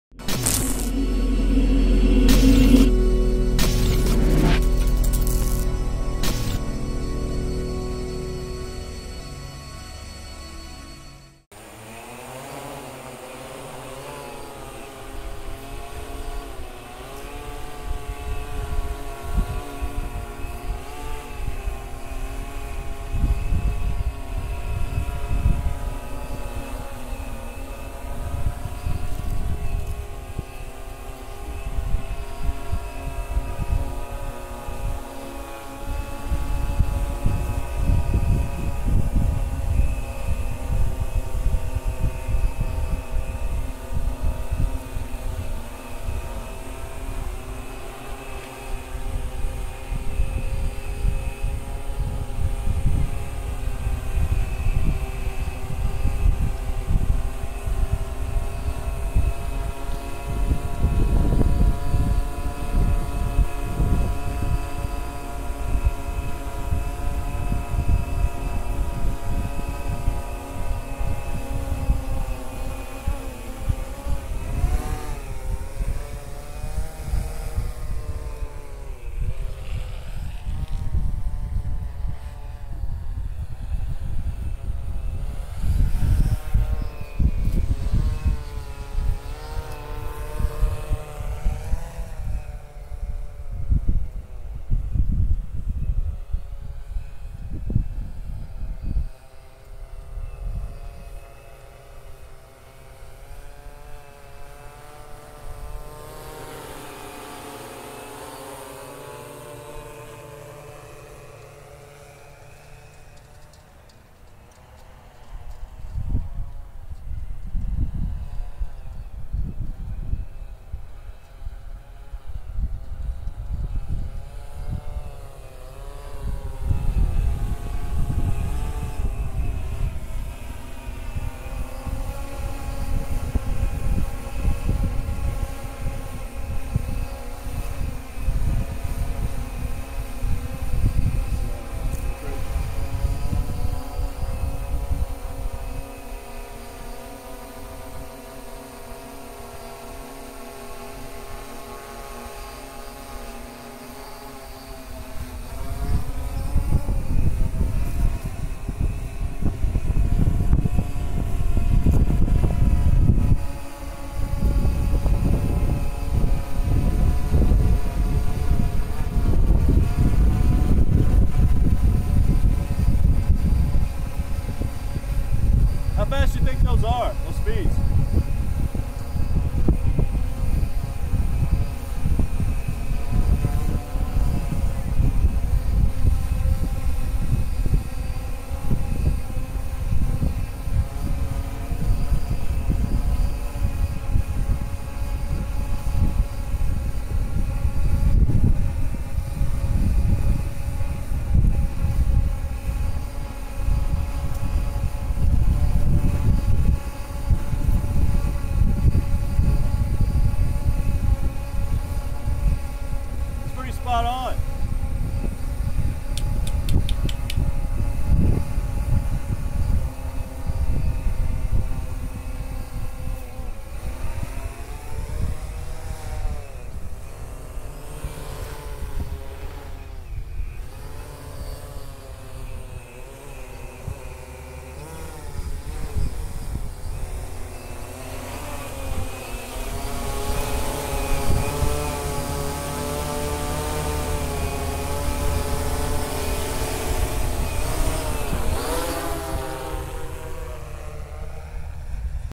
WhiteBoxStudios_Custom_Y6_Test
Wind speeds for Sunday February 8, 2015 for zip code 35630 were between 20 and 25 mph.  Not bad positioning for first flight.